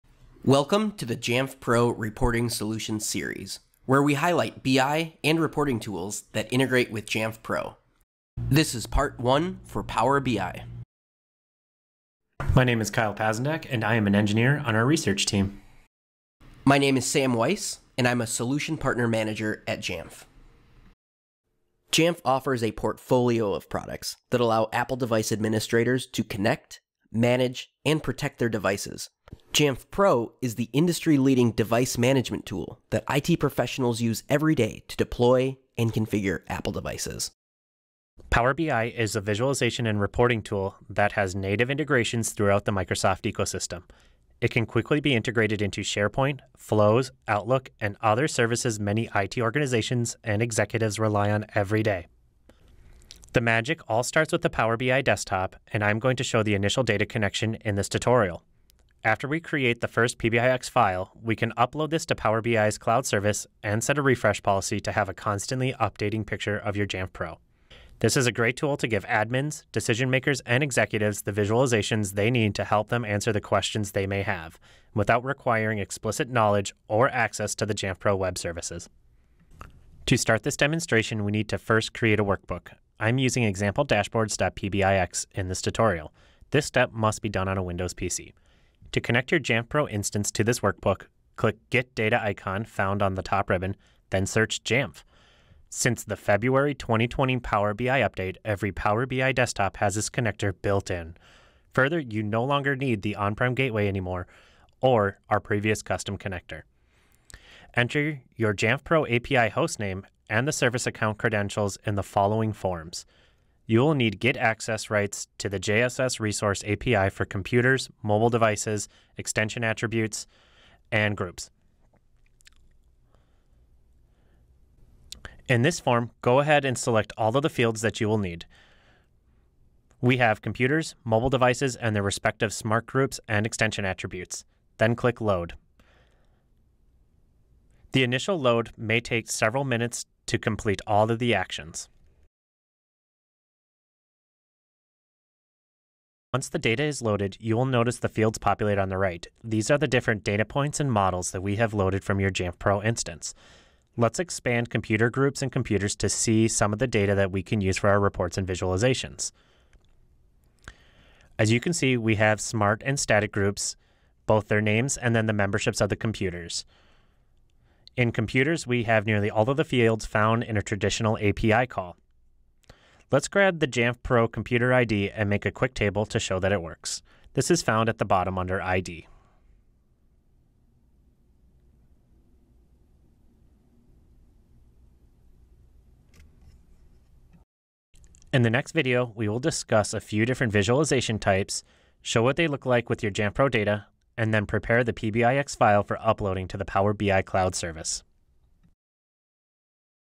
Power BI: Module 1 | Jamf Pro Reporting Solutions Series
Part one covers creating a workbook and authenticating to Jamf Pro as a data source.

Power BI is Microsoft’s reporting and visualization engine that has native integrations throughout the Microsoft ecosystem. Recent updates have made it easier than ever to connect Jamf Pro to Power BI as a data source. This series is an introduction to integrating these two systems to provide a robust dashboard for Jamf administrators.

Learn more about Power BI, and other Jamf integrations on the Jamf Marketplace